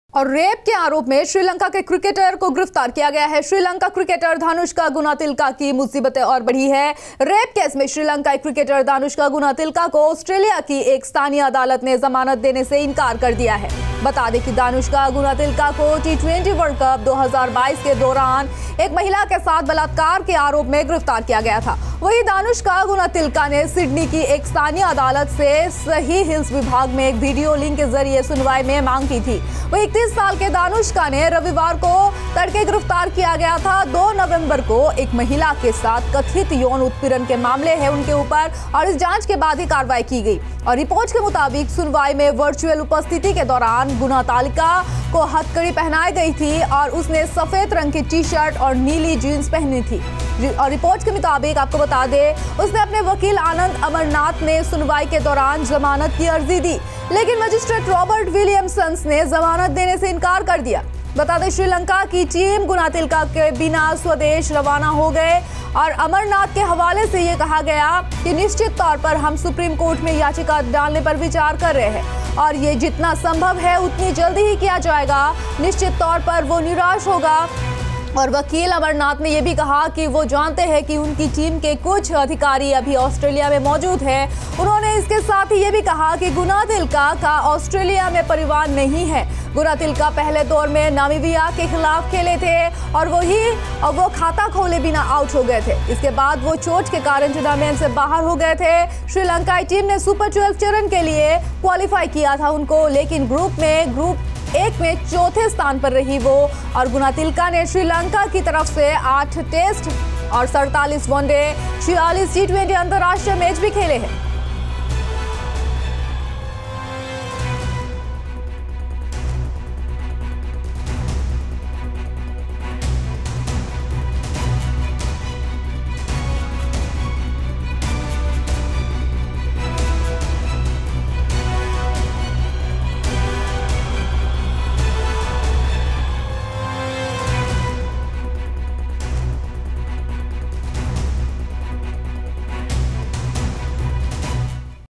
RAPE के आरोप में श्रीलंका क्रिकेटर गिरफ्तार
RAPE के आरोप में
श्रीलंका क्रिकेटर गिरफ्तार